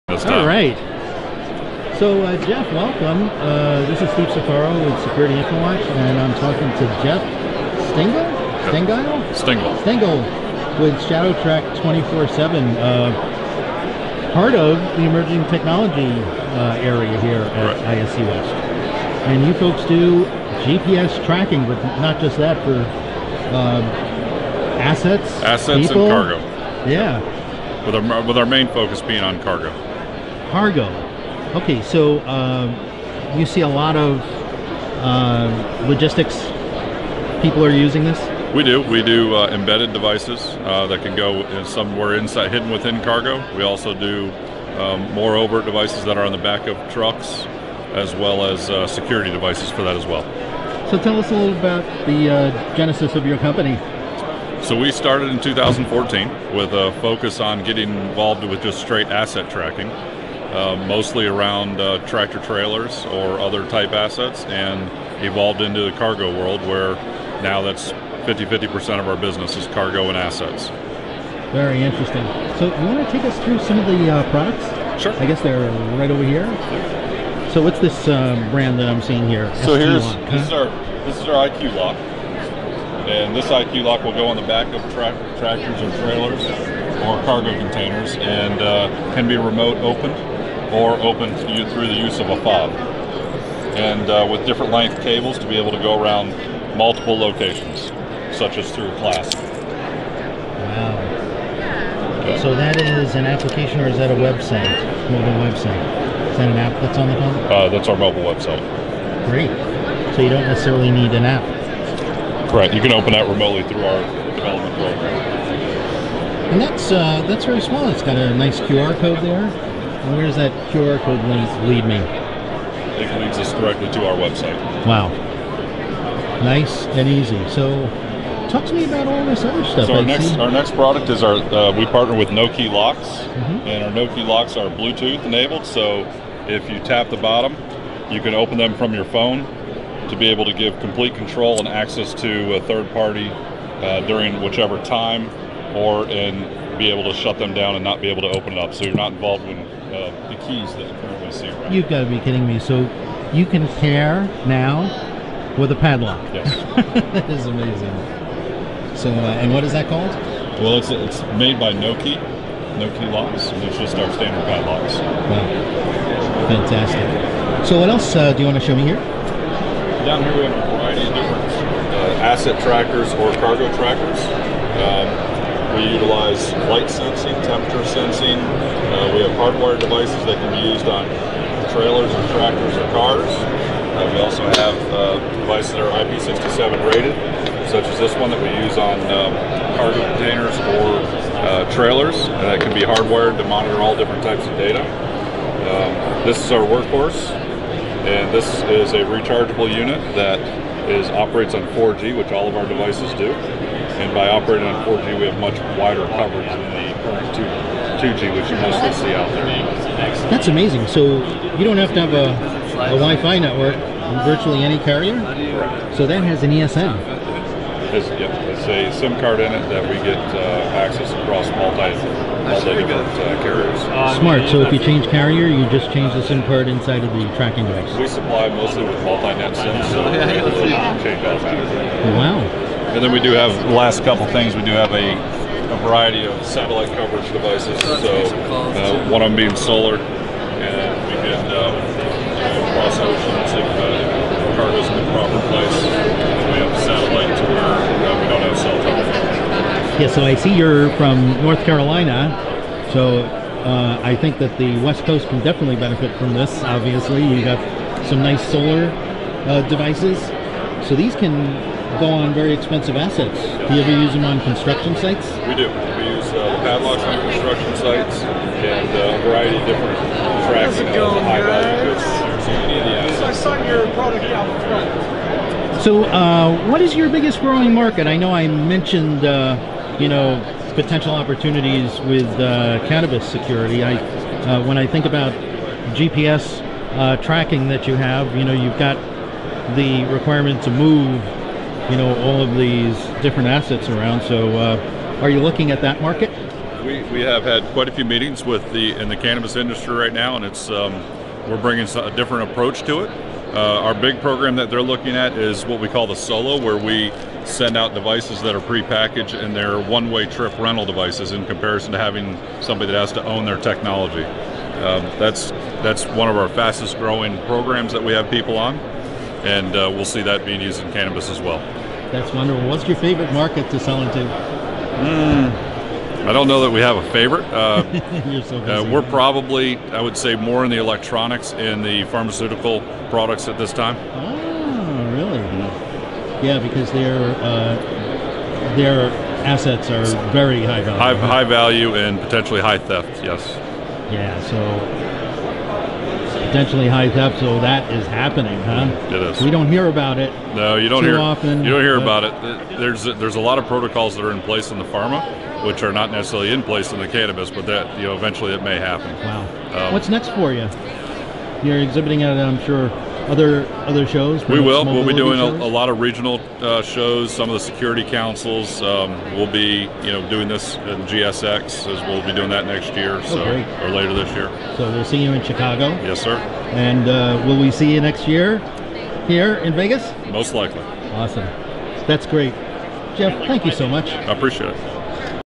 ShadowTrack 24/7 makes asset and cargo tracking smarter
SecurityInfoWatch contributor Steve Surfaro talks with Jeff Stingel, President of ShadowTrack 24/7, about the company’s range of GPS tracking solutions for mobile and fixed assets and how they are being leveraged in the market at ISC West 2019.